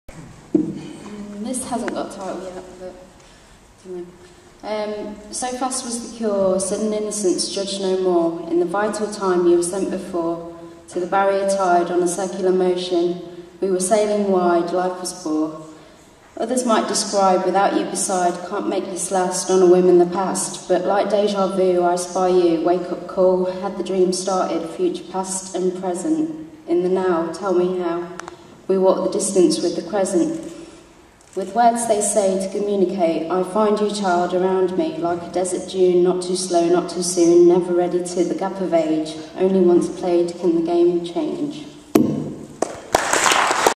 WORD!@Everybody's Reading 9
See performers from WORD! open mic poetry night during the Everybody's Reading Festival in Leicester supported by Whatever it Takes.